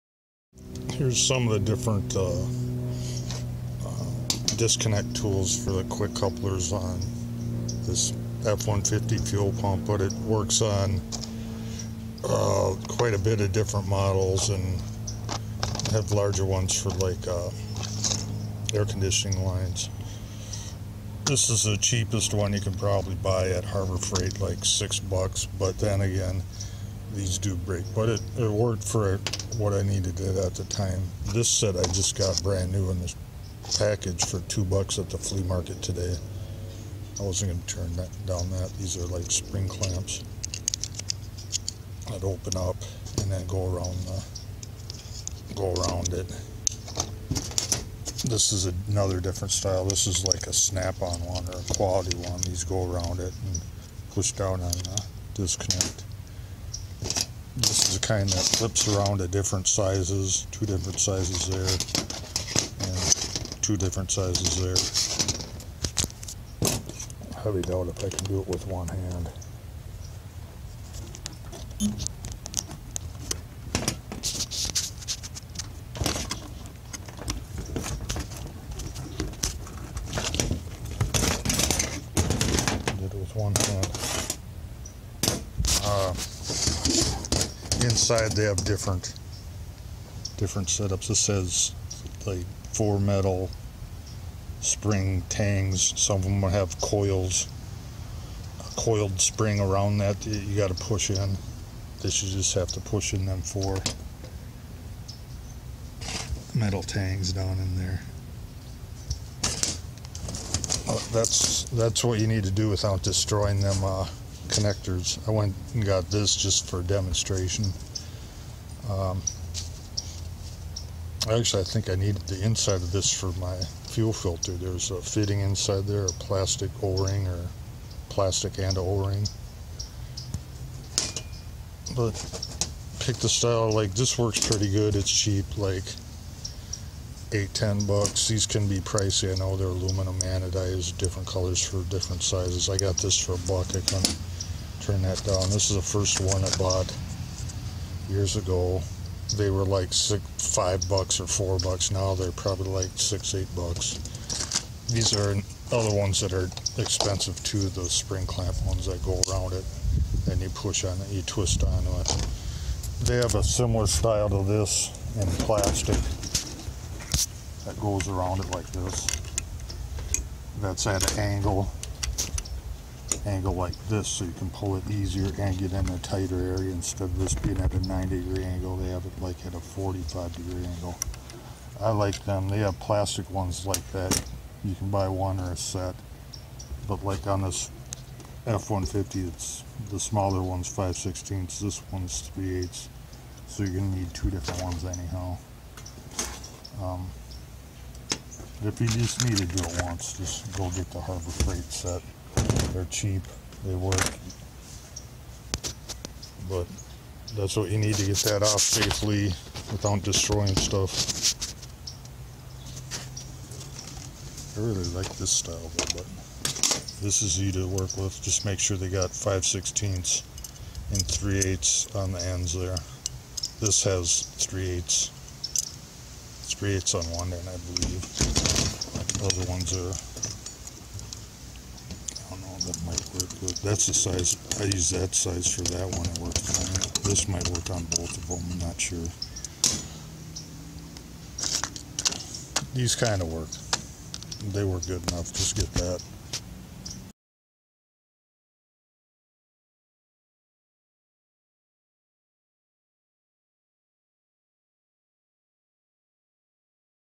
Which Quick Disconnect Coupler Tool is Best?
5/19/22 Update - I bought the $4.99 Amazon blue plastic angled disconnect tools. The delivery date changes to 16 days. Ordered 5/1/22 arrived today, 3 days late. Look really good for the price. I don't need them right now , but they will come in handy!

The set of Quick Disconnect Coupler Tools was on Amazon and shipping was over 60 days from China -even though it doesn't state it in the listing.
 I may buy later if it's still on sale.
 I won't provide a link, because price and link can/will change.

The Redneck Tech
For more car builds, old stored car starts, tools and product reviews!